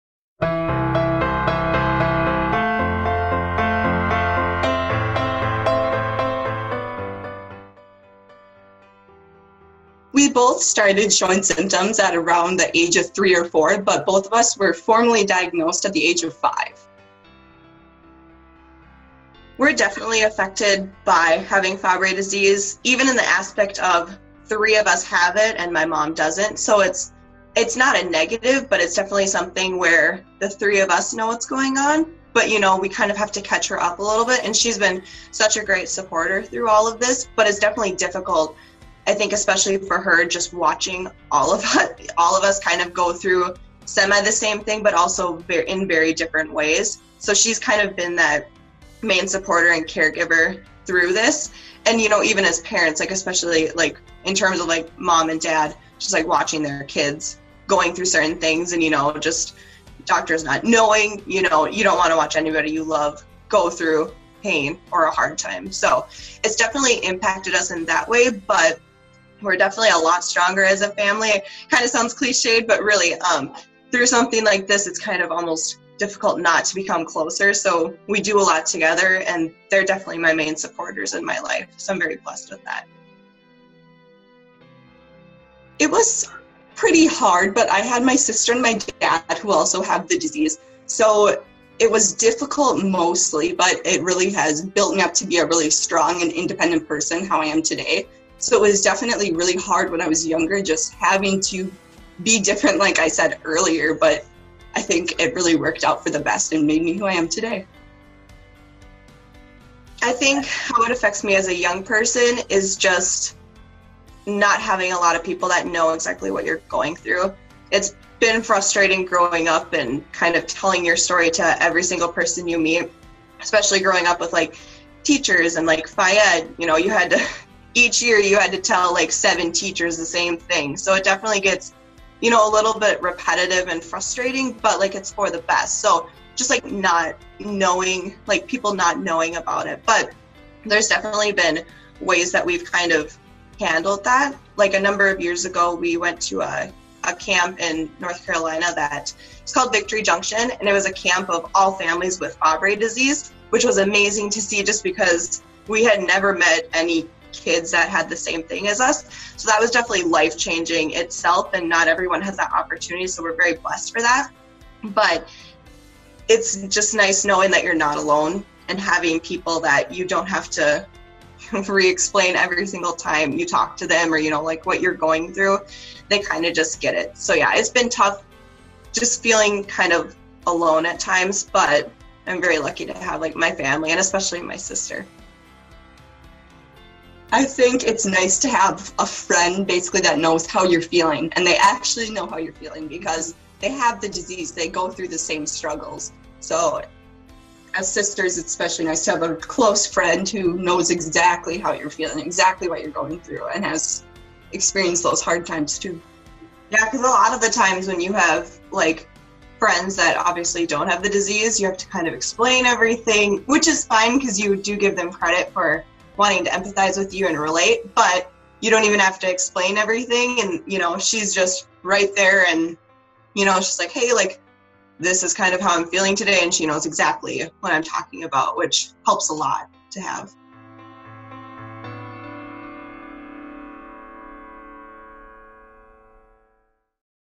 Growing up with Fabry disease
Stephanie and Sarah have Fabry disease, and have been raising awareness in the Fabry disease community. They shared with us their experiences of living with a rare disease.